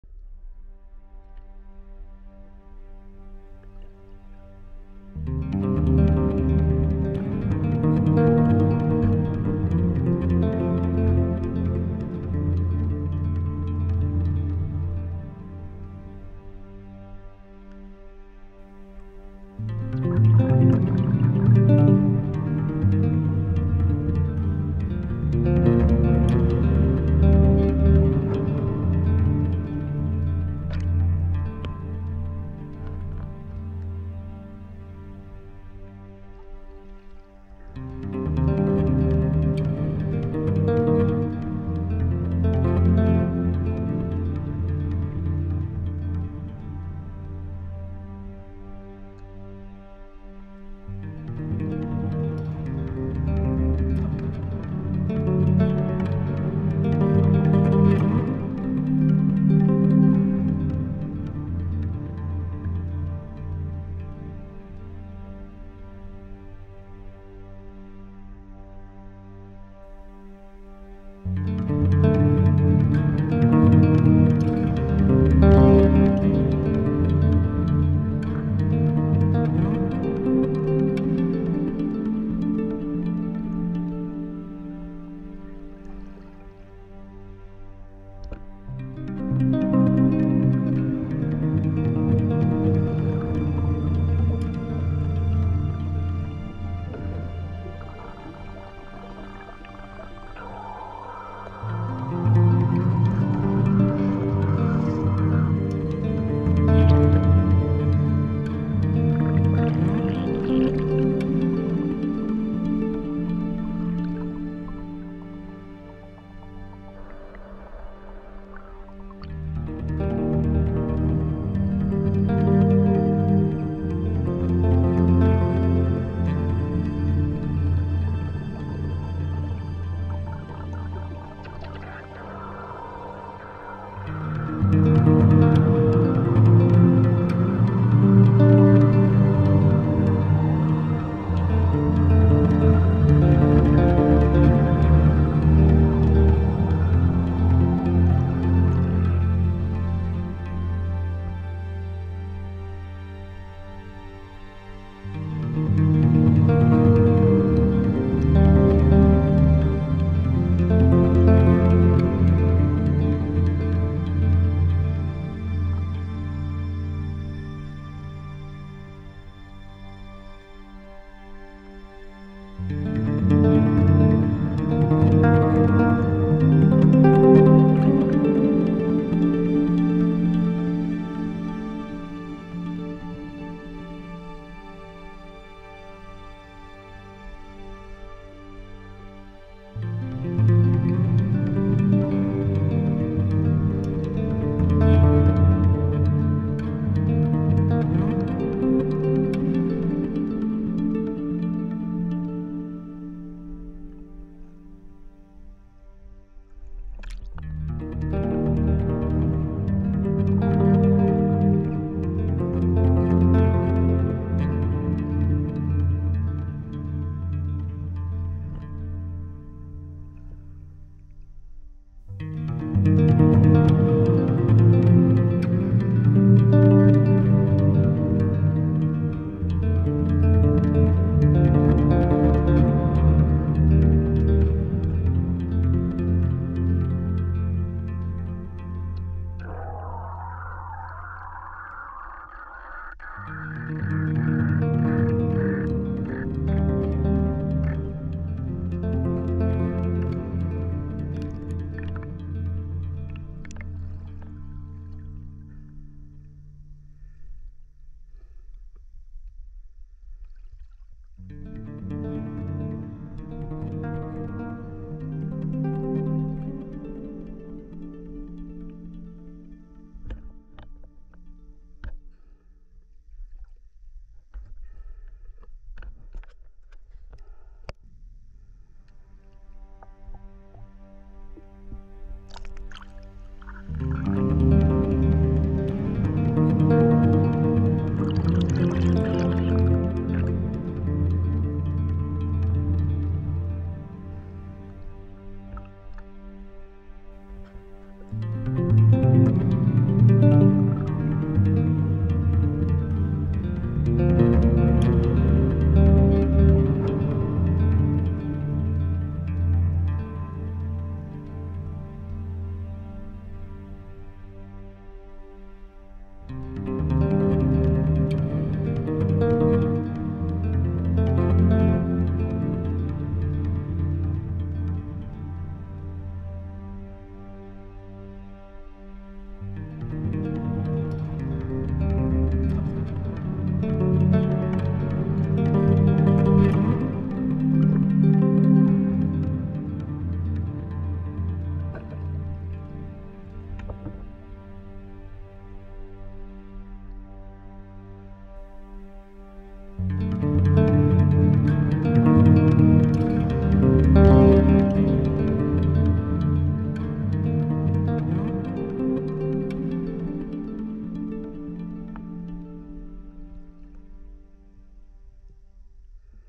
Tauchen Kiesgrube Gremberghofen
Wunderschöne Unterwasserwelt in einem See. Flussbarsche und Blässhühner. Sehr klares Wasser